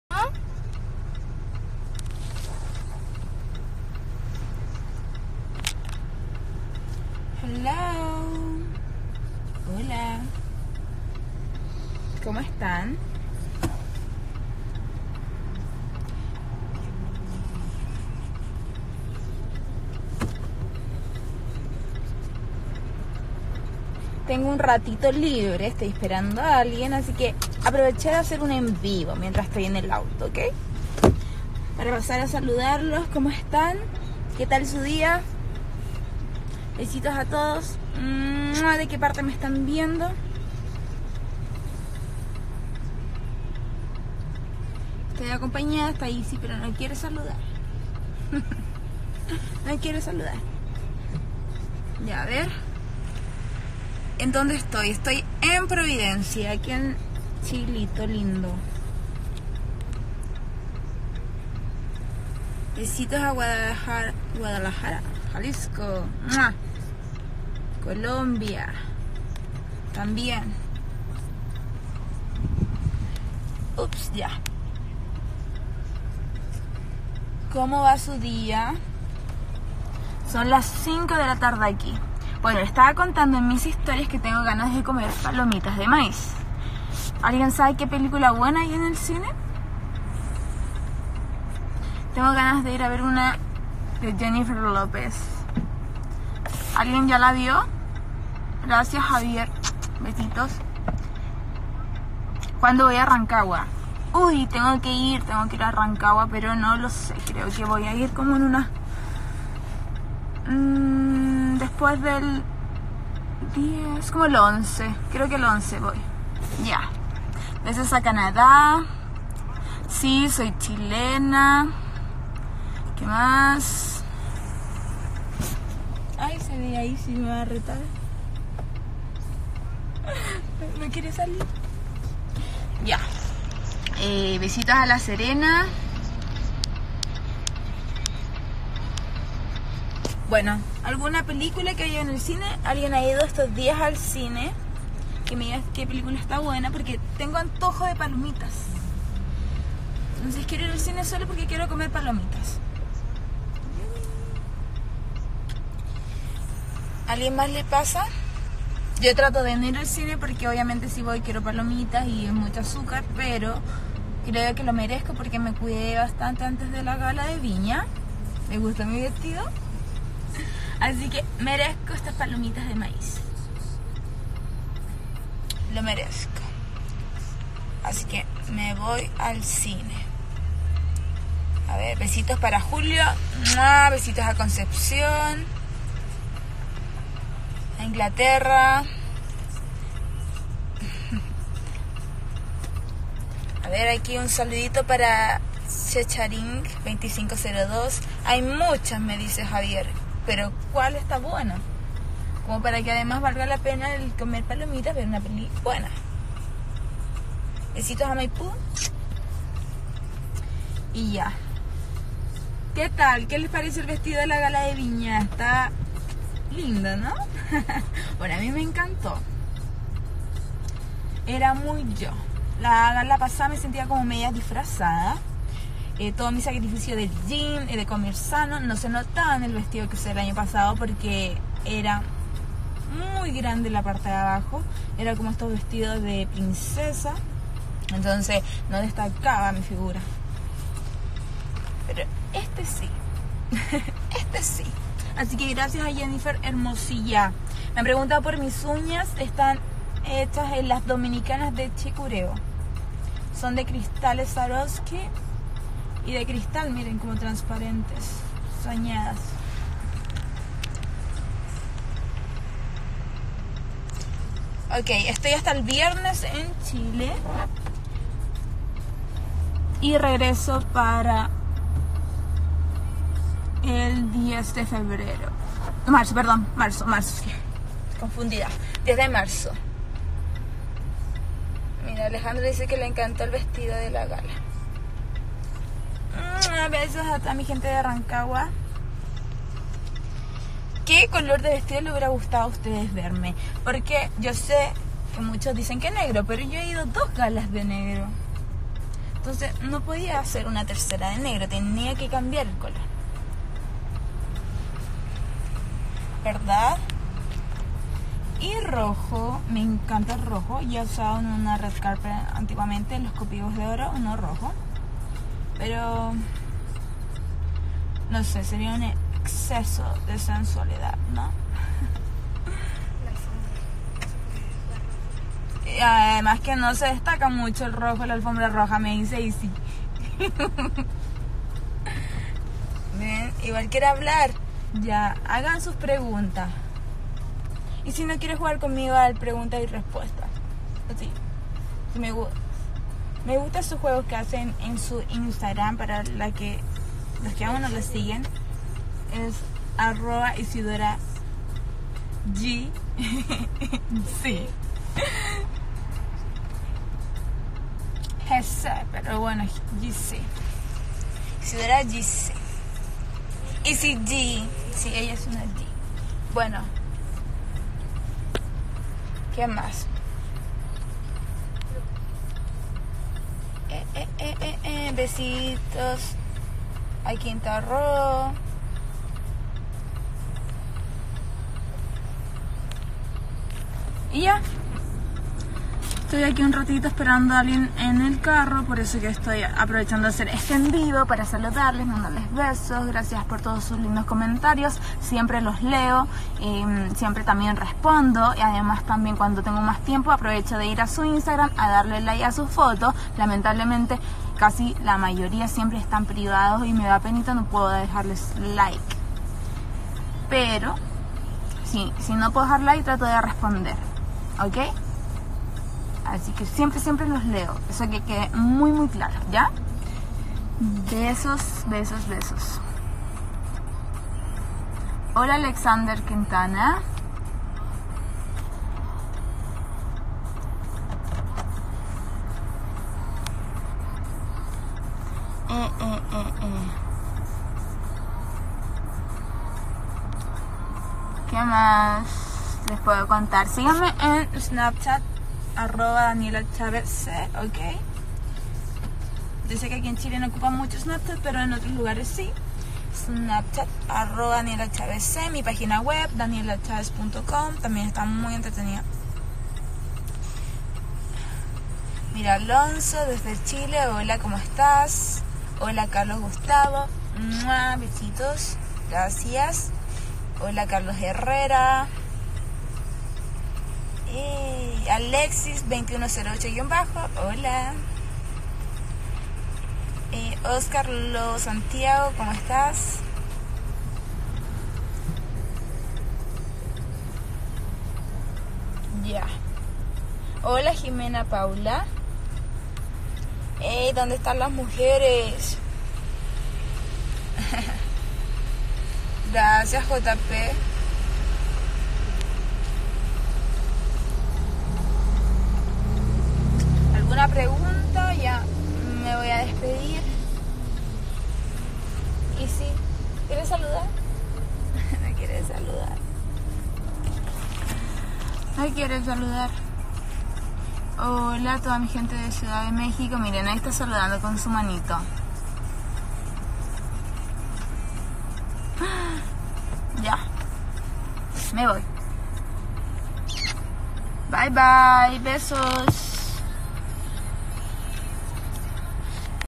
Daniella Chavez - Instagram Live 26 De Febrero De 2019
Grabación integra de la transmisión que hizo desde su cuenta oficial de instagram la actriz y modelo chilena Daniella Chavez el pasado 26 de Febrero de 2019.